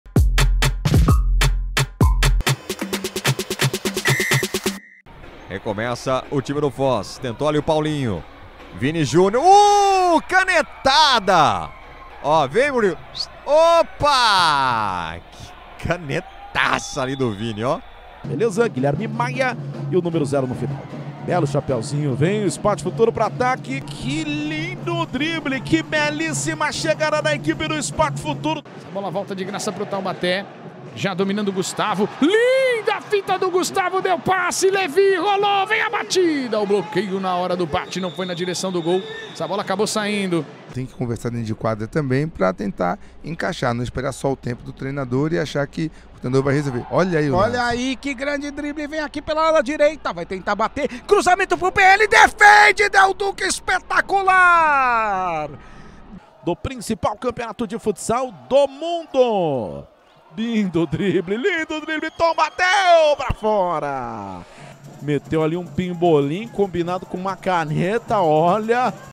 TOP 5 DRIBLES | 21ª RODADA | LNF 2024
Nossa equipe selecionou os TOP 5 dribles da 21ª rodada da LNF 2024!

Curtiu? Faltou algum? Comenta aí!

📽️LNFTV